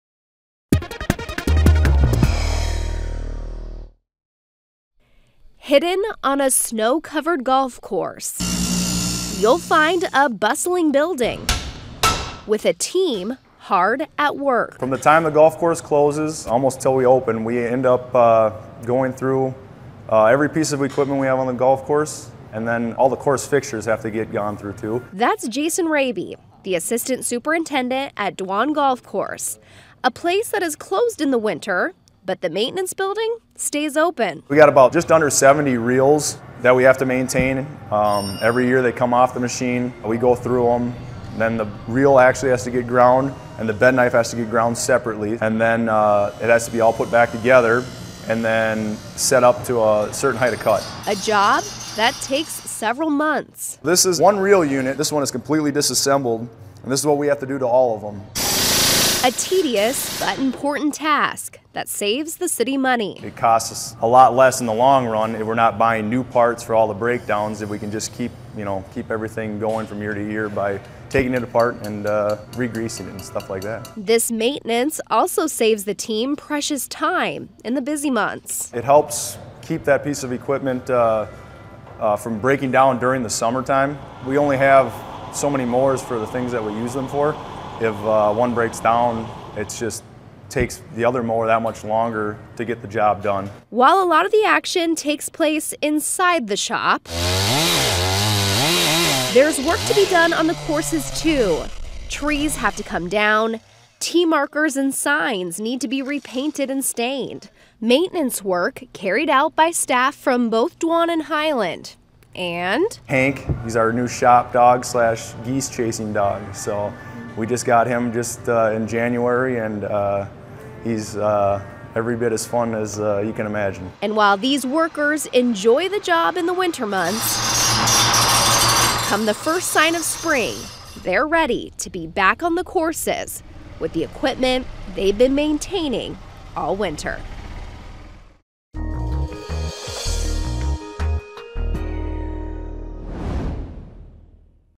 What goes on at Dwan Golf Course during the winter?
Dwan and Hyland golf courses might be closed during the winter, but the work doesn’t end with the snow.